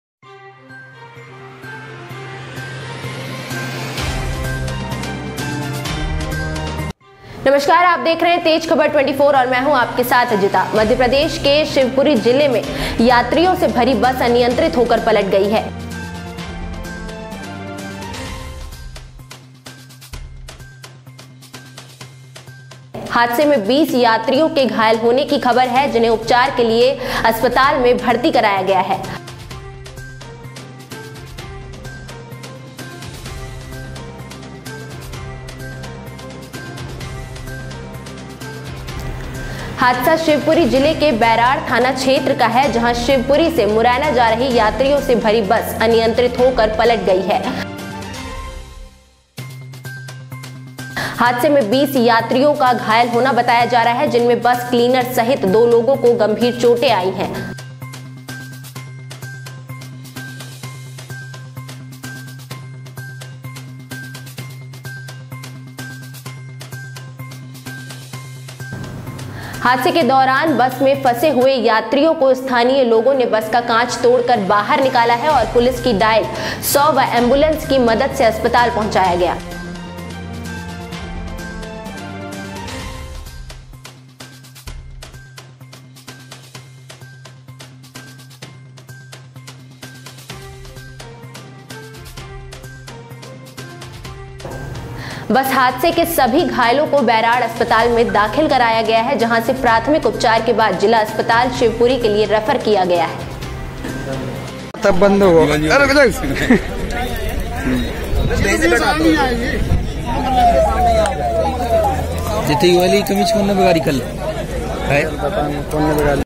यात्रियों से भरी बस पलटी, 20 यात्री हुये घायल 2 की हालत गंभीर
Tezkhabar24_08

Tezkhabar_07

हमारी वेबसाइट

Twitar पेज